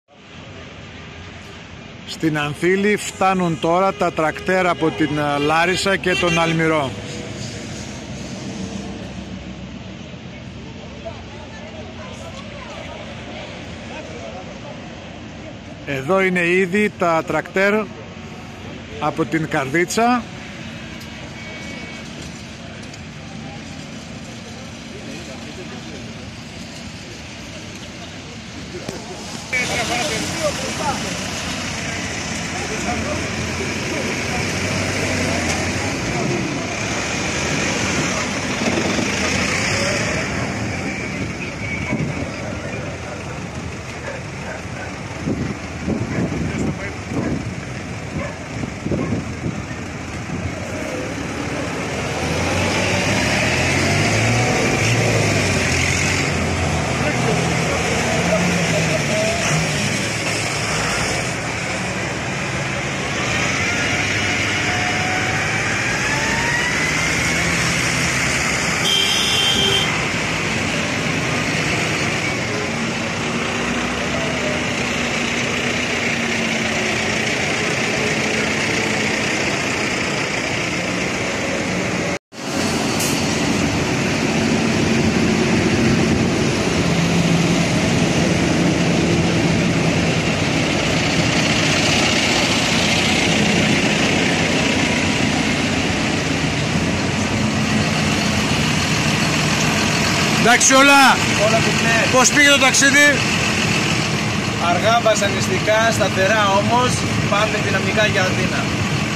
Στην Ανθήλη συναντήθηκαν τα τρακτέρ από Καρδίτσα, Λάρισα και Αλμυρό
Στην Ανθήλη συναντήθηκαν τα τρακτέρ από Καρδίτσα, Λάρισα και Αλμυρό. Σε λίγο αναχωρούν όλοι μαζί για Κάστρο Βοιωτίας.

Νωρίτερα έγινε θερμή υποδοχή των αγροτών της Καρδίτσας στο Μπλόκο της Ανθήλης.